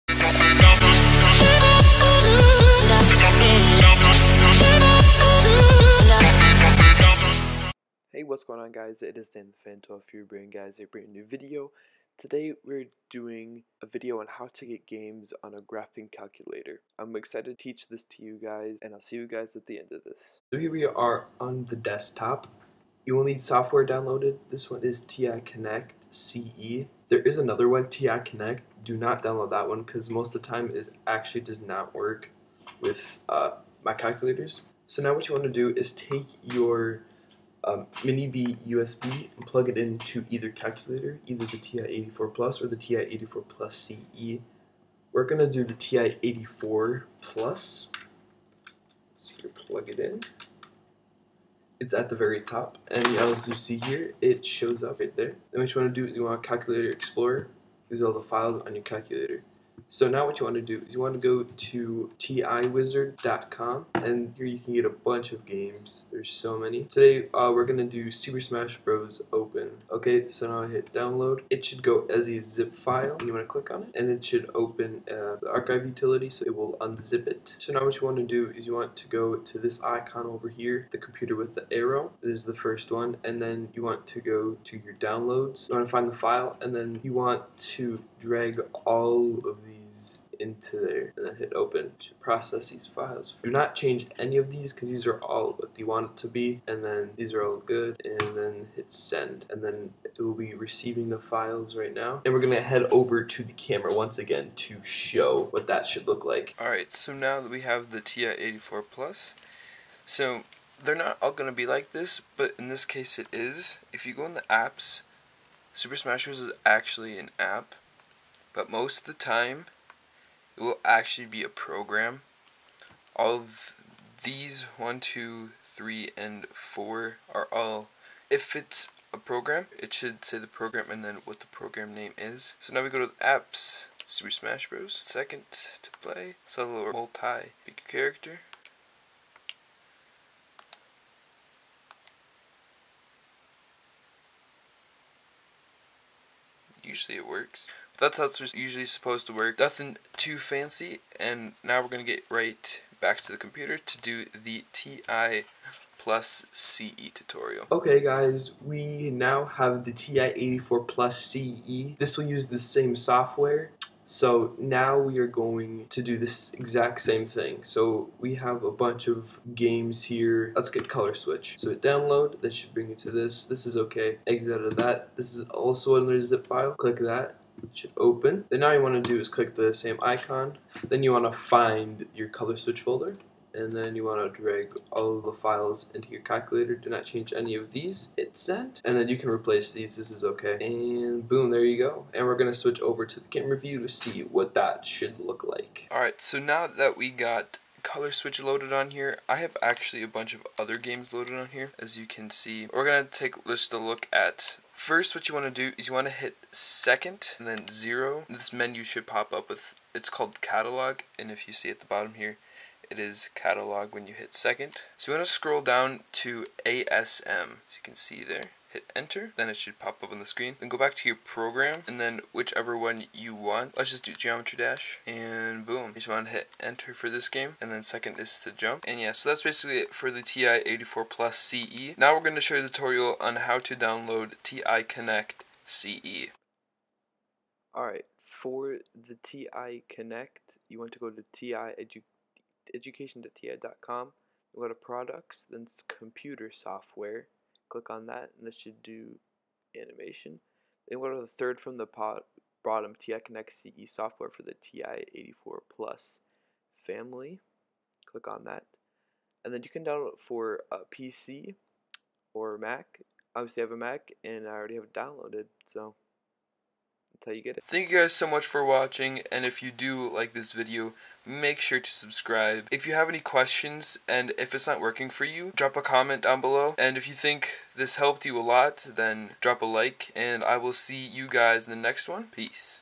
GAMES ON GRAPHING CALCULATOR!!!! *Tutorial*
How to get games on a graphing calculator. This process should work for all calculators with a Mini-B USB type.